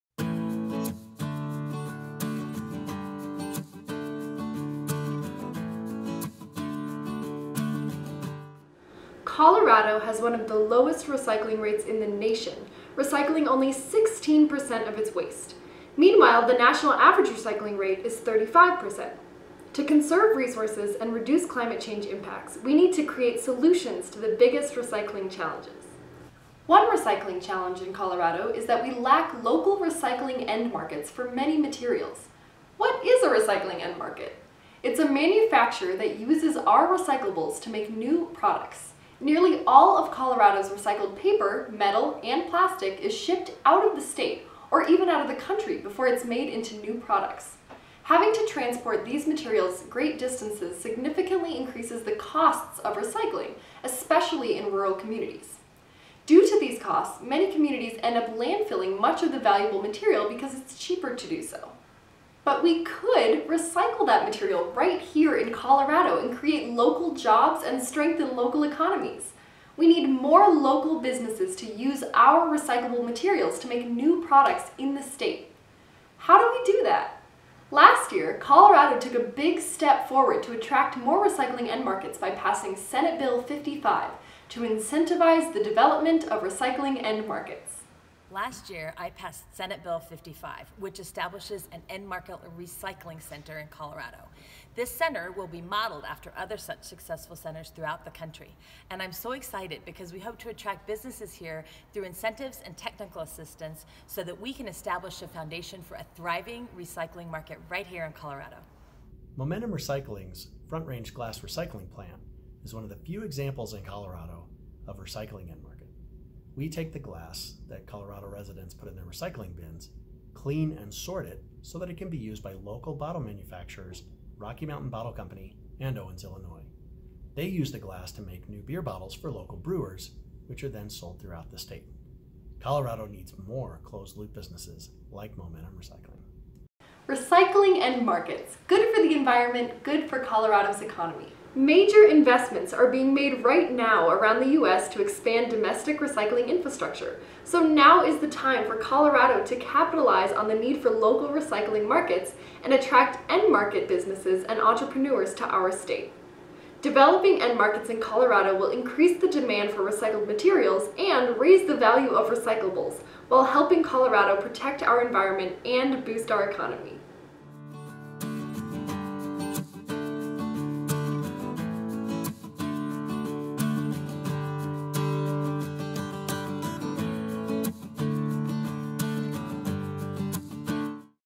Developing Recycling End Markets in Colorado
Now is the time for Colorado to capitalize on the need for local recycling markets and attract end market businesses and entrepreneurs to our state. Developing end markets in Colorado will increase the demand for recycled materials and raise the value of recyclables, while helping Colorado protect our environment AND boost our economy! To learn more about the importance of developing recycling end markets in Colorado, read our new report, The State of Recycling and Composting in Colorado, which was created with the help of our friends at CoPIRG

Special thanks to Representative Cutter and John Lair from Momentum Recycling for taking part in this video!